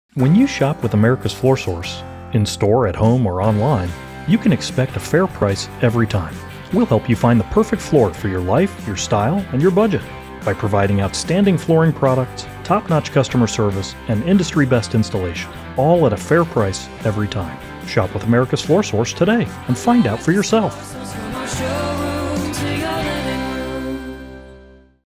Fair Price Promise :25
From the moment you step into our showroom – or your Personal Flooring Advisor walks into your living room – our goal is to make sure you get the perfect floor for you, your home, your family, and your budget.

And that's why we provide outstanding flooring products, industry-best installation, and top-notch customer service – all at a fair price, every time.